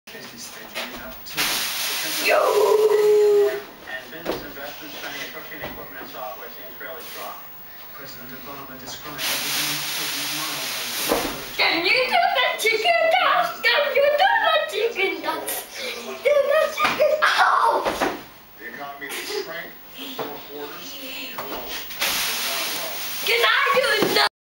I live in a slapstick film
Watch Henry take a dive then pop up in 3 seconds...